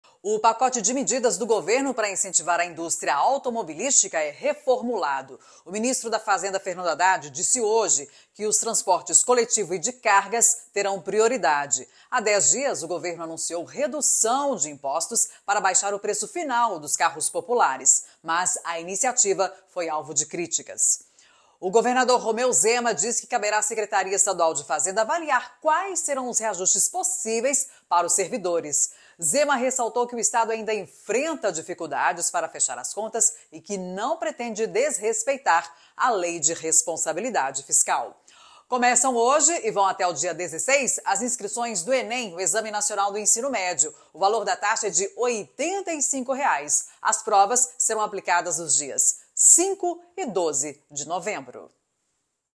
INSCRIÇÃO DO ENEM, ROMEU ZEMA E FERNANDO HADDAD | AS PRIMEIRAS DO DIA, POR KÁTIA PEREIRA
1️⃣ O pacote de medidas para incentivar a indústria automobilística é reformulado. O ministro da Fazenda, Fernando Haddad, disse nesta segunda-feira (5) que os transportes coletivos e de cargas terão prioridade.  Há 10 dias, o Governo anunciou redução de impostos para baixar o preço final dos carros populares. Mas a iniciativa foi alvo de críticas.

2️⃣ O governador Romeu Zema diz que caberá à Secretaria de Estado de Fazenda avaliar quais serão os reajustes possíveis para os servidores. Zema ressaltou que o estado ainda enfrenta dificuldades para fechar as contas e que não pretende desrespeitar a Lei de Responsabilidade Fiscal.

3️⃣ Começam nesta segunda-feira, e vão até o dia 16 as inscrições para o Exame Nacional do Ensino Médio (Enem) deste ano. O valor da taxa é de R$ 85,00 e as provas serão aplicadas nos dias 5 e 12 de novembro.

📹 Guilherme Reis | Itatiaia